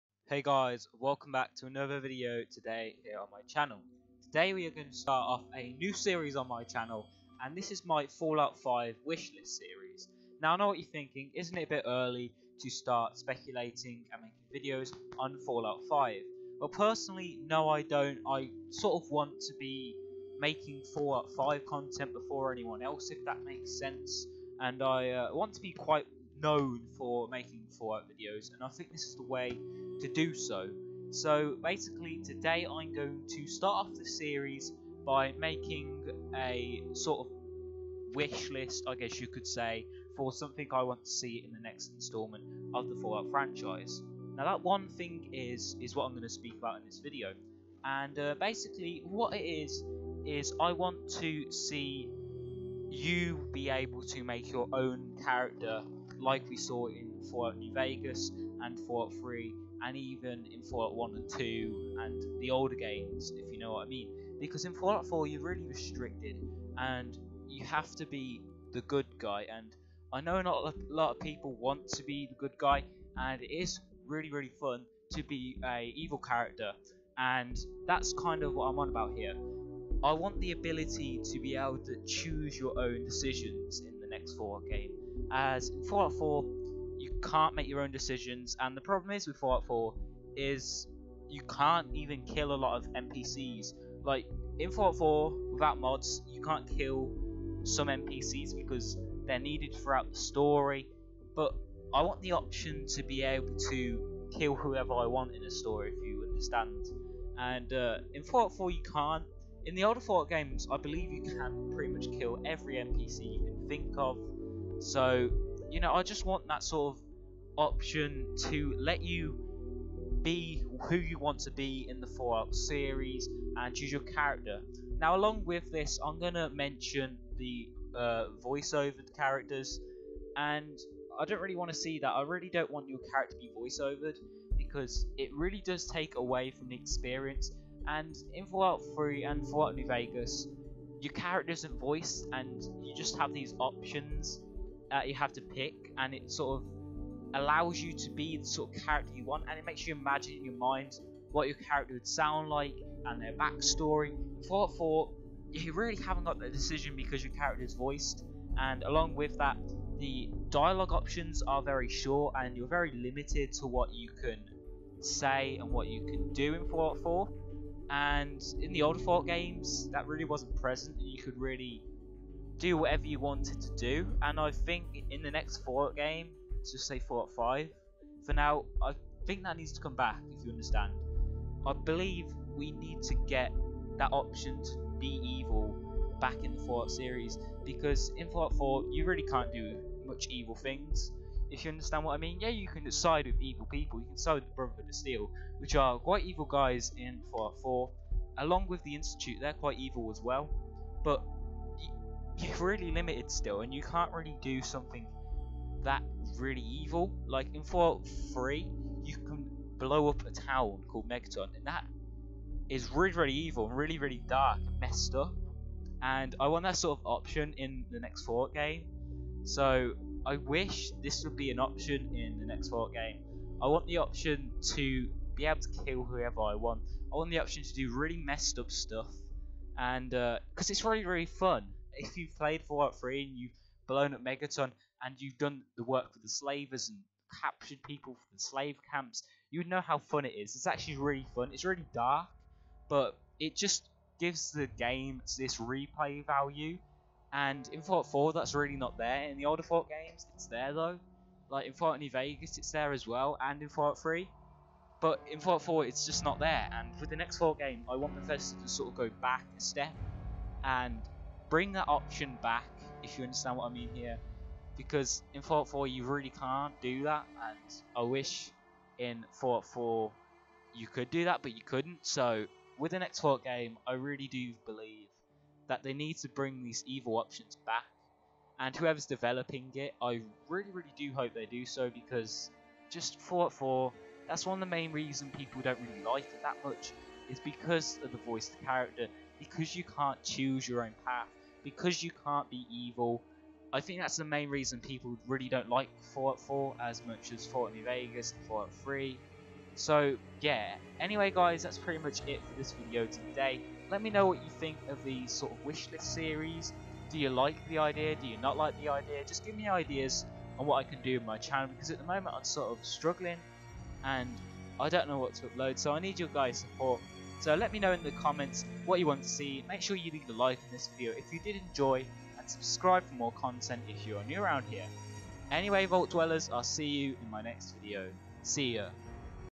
Fallout 5 wishlist: The option to be evil
Hi guys in this video I will be sharing one of my wishes for the next Fallout game which at the moment we don't know much about therefore all of this video is just speculation and hasn't been confirmed by anyone!